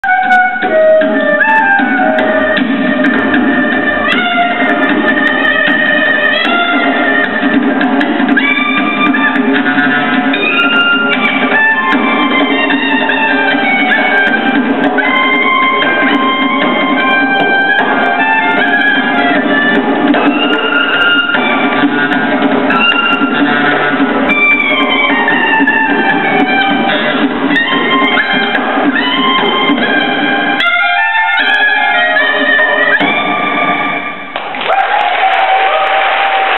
Lerner Moguilevsky Duo
Lerner Moguilevsky Duo from Buenos Aires at the 17th Jewish Culture Festival in Cracow